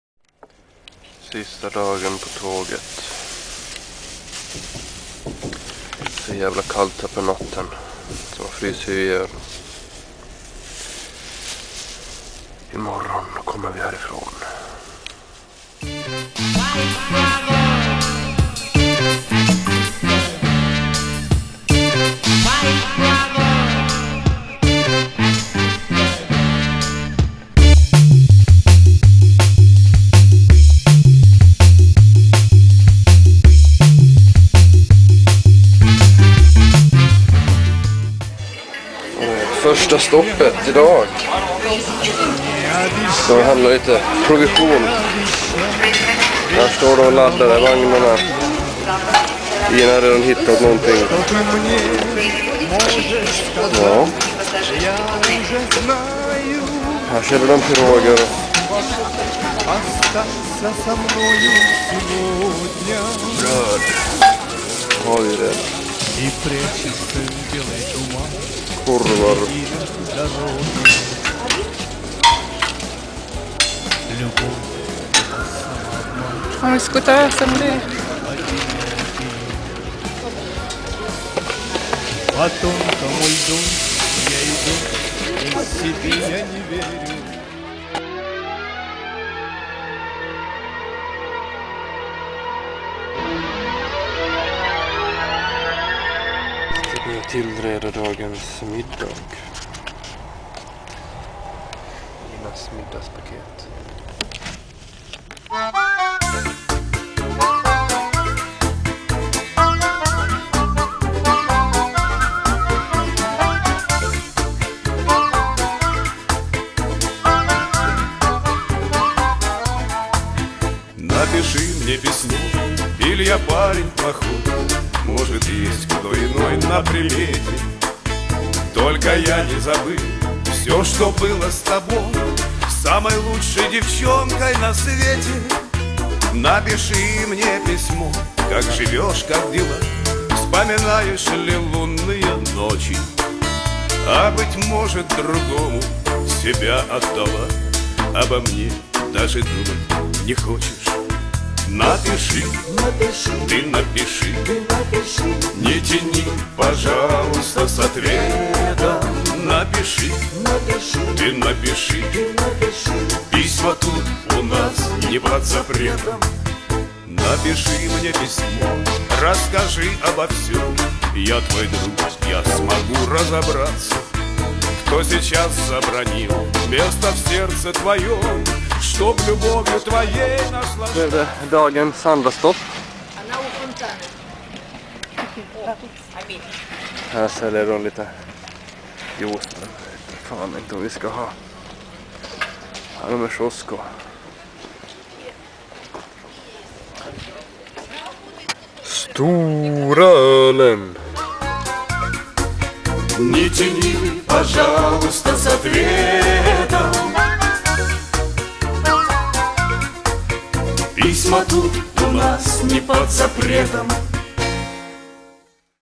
Stenen i Vattnet - Dag 008
Sista dagen på tåget före Irkutsk.